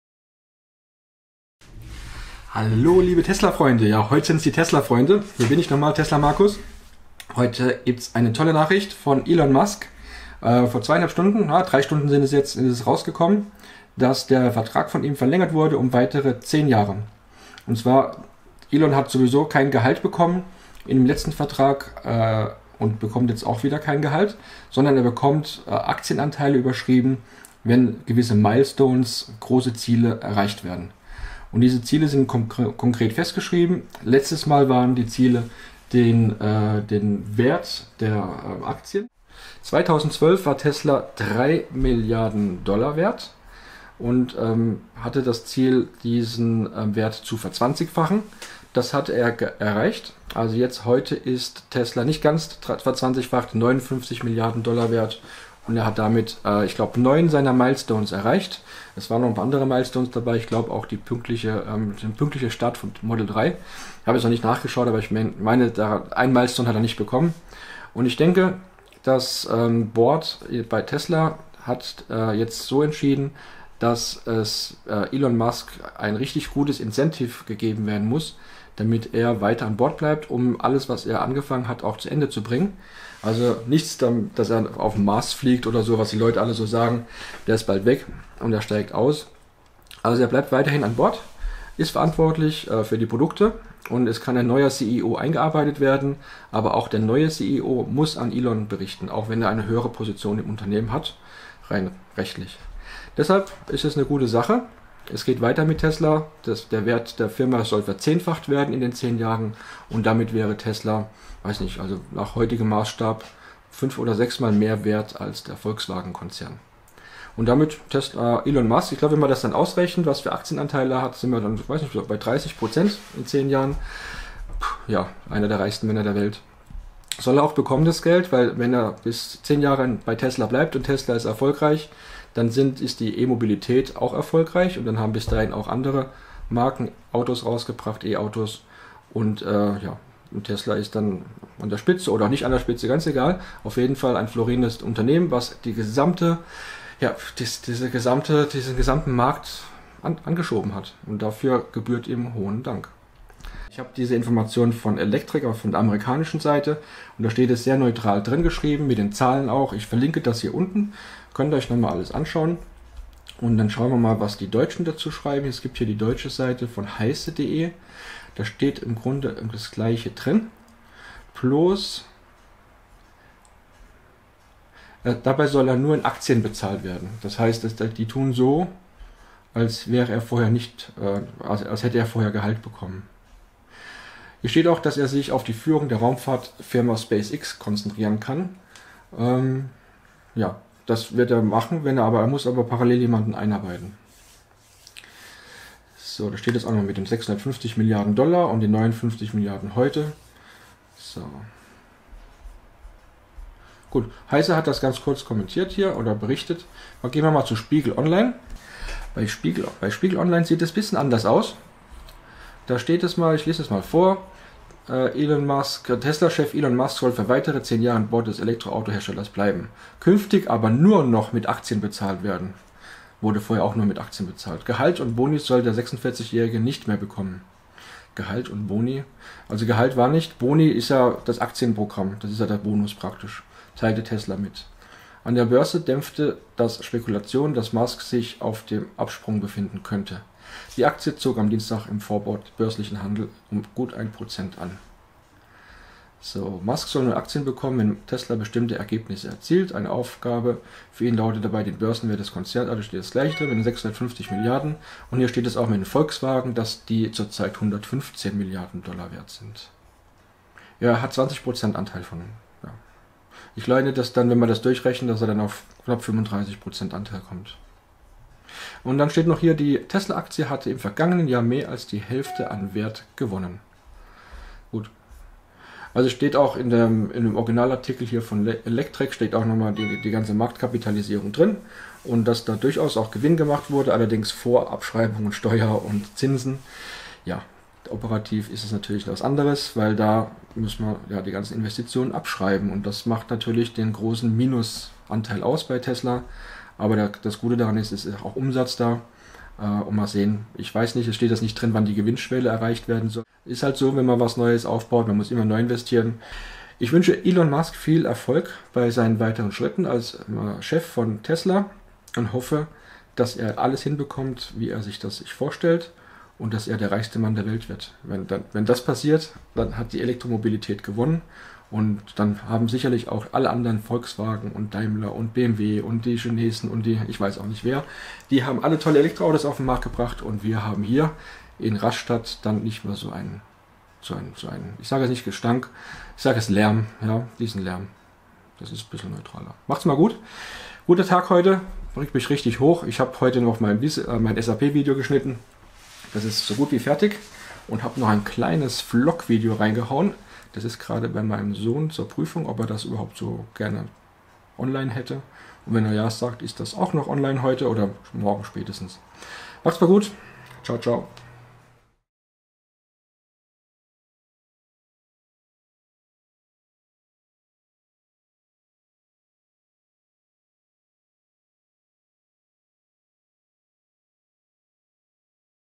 Ich gratuliere: Elon Musk bleibt für weitere 10 Jahre bei Tesla!
Heute, am 23.01.18 gibt es eine Pressemitteilung zur Vertragsverlängerung von Elon Musk bei Tesla.

Elon Musk verzichtet weiterhin auf Gehalt und läßt sich anhand der Erreichung von Milestones bezahlen.